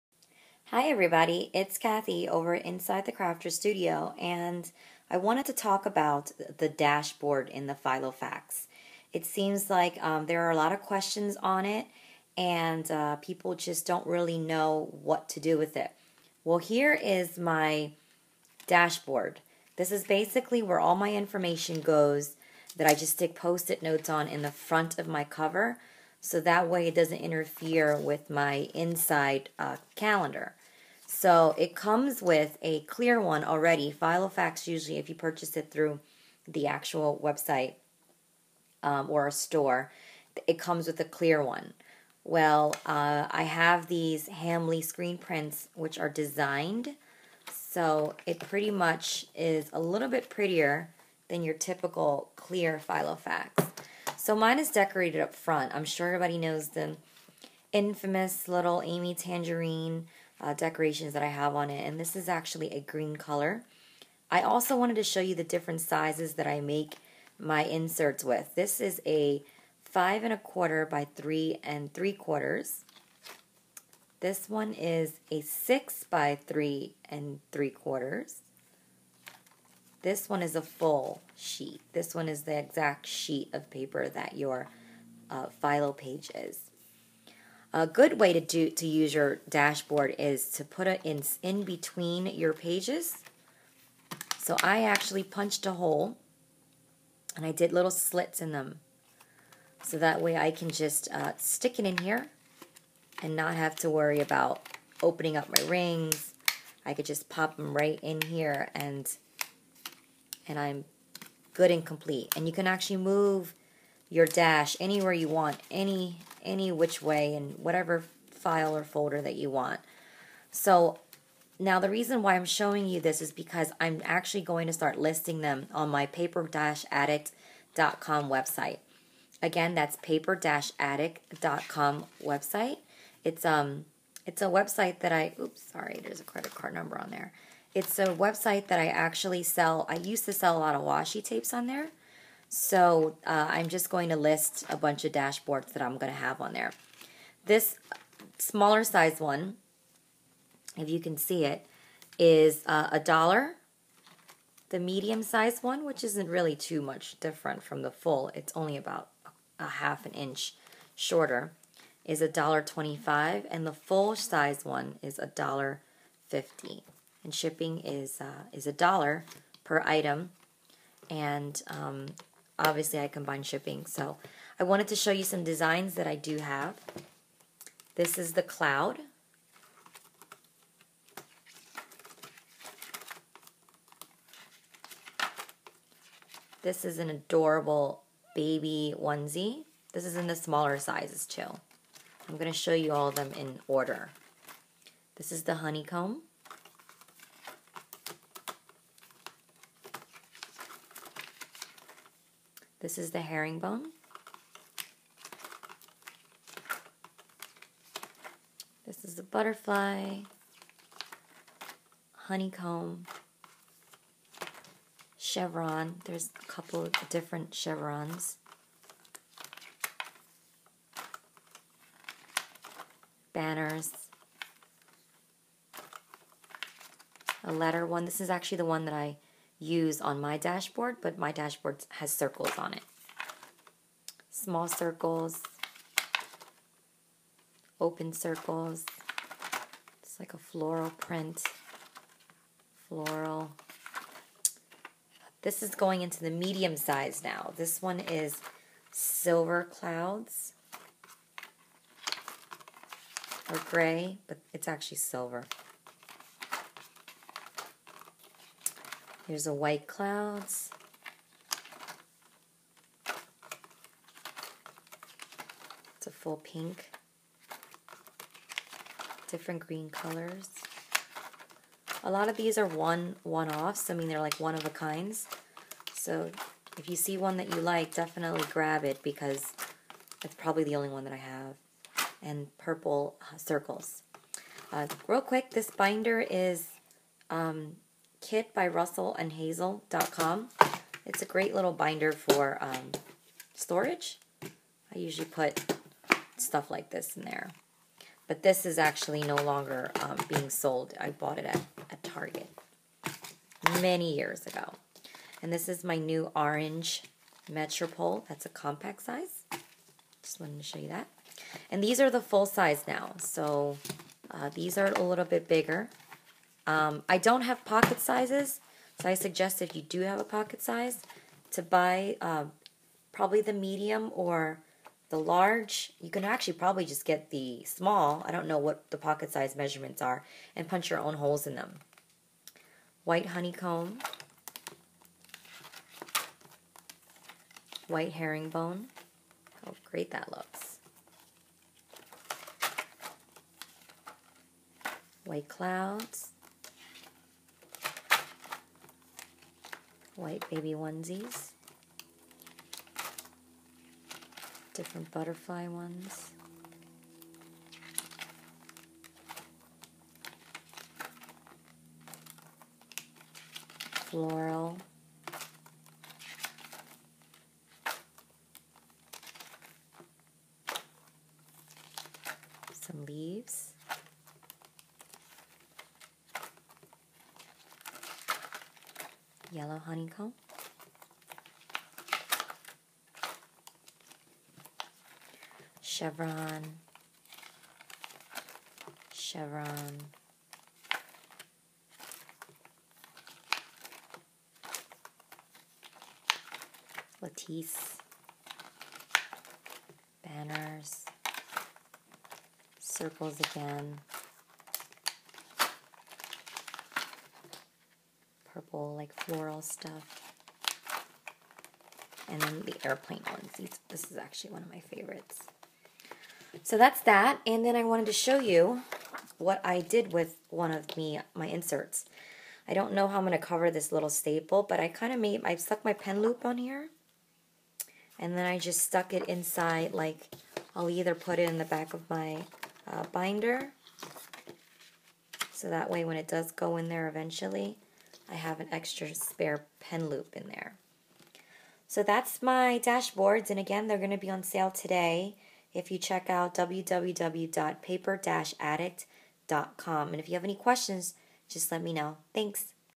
Dashboard by Hambly Screenprints for Filofaxes
Since I have a neverending supply of Hambly screen print overlays, why not help my fellow Filofaxes beautify their Filofaxes with these cute dashboards!

Comes in 3 sizes to fit the personal, looks beautiful stacked! Time to use up those post it notes!